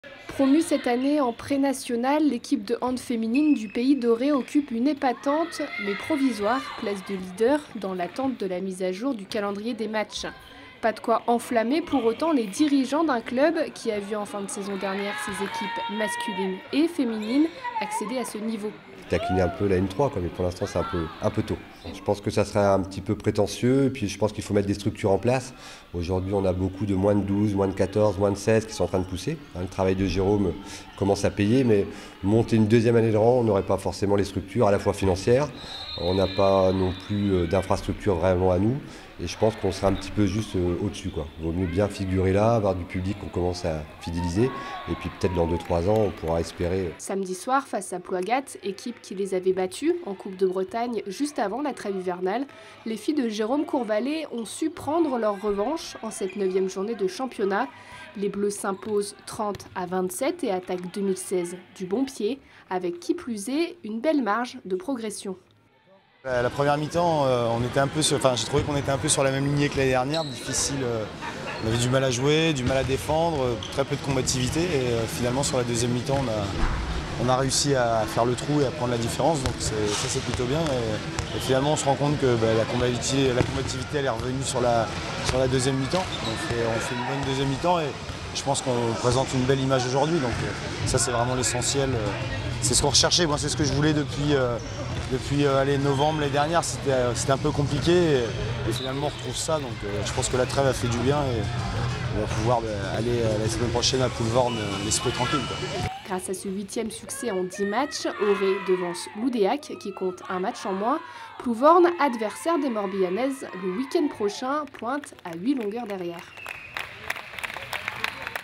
Handball (Prénationale F) - Le PAHB épatant (provisoire) leader
- Reportage de la rédaction des sports de TébéSud -
Promue cette année en prénationale, l'équipe de hand féminine du pays d'Auray occupe une épatante - mais provisoire - place de leader, dans l'attente de la mise à jour du calendrier des matches.
Pas de quoi enflammer pour autant les dirigeants d'un club qui a vu en fin de saison dernière ses équipes masculine et féminines accéder à ce niveau.